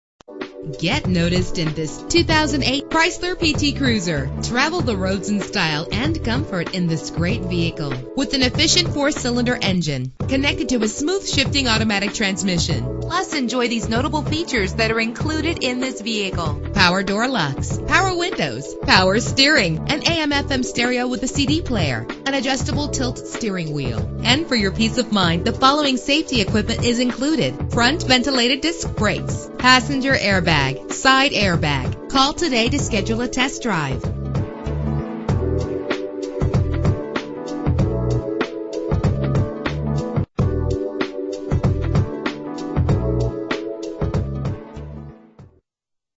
2008 Chrysler PT Cruiser - Temple Hills MD
Year: 2008
Make: Chrysler
Model: PT Cruiser
Trim: LX
Engine: 2.4 liter inline 4 cylinder 16 valve
Transmission: Automatic
Color: Sunset Crystal
Mileage: 35042
Address:
4533 St Barnabas Rd